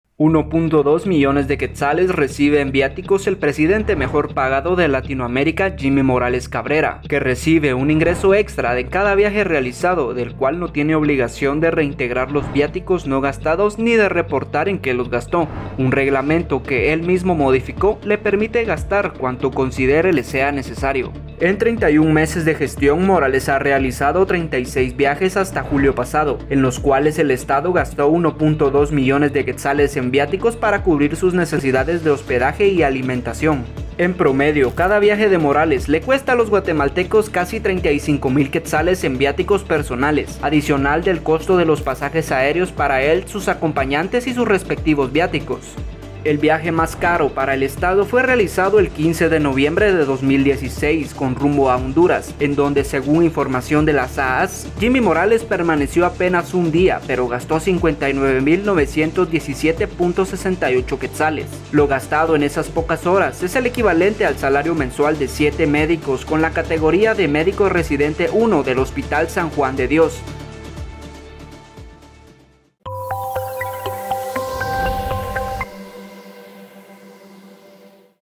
Q1.2 millones en viáticos para Jimmy Morales
El presidente mejor pagado de Latinoamérica recibe un ingreso extra en cada viaje realizado, del cual no tiene obligación de reintegrar los viáticos no gastados ni reportar en qué los gastó. Un reglamento que él mismo modificó le permite gastar cuanto considere le sea necesario.